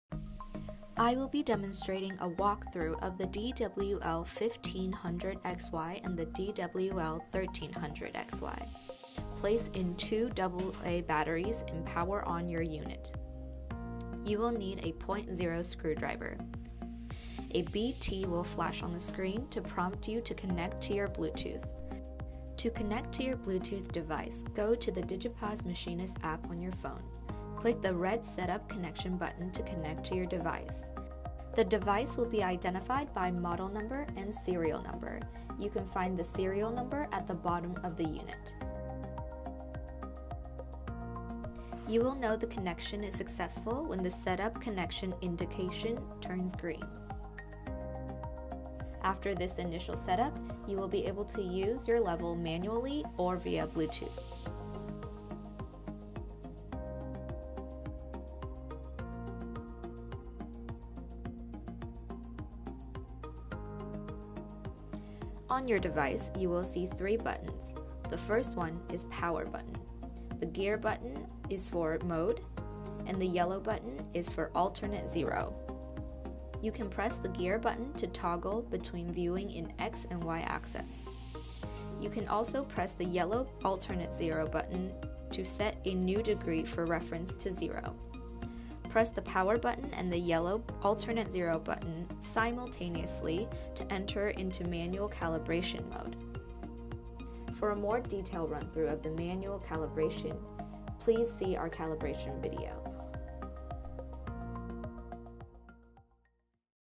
Demonstration Video for DWL-1300XY & DWL-1500XY
Demonstration walkthrough for DWL-1300XY and DWL-1500XY Digital Machinist level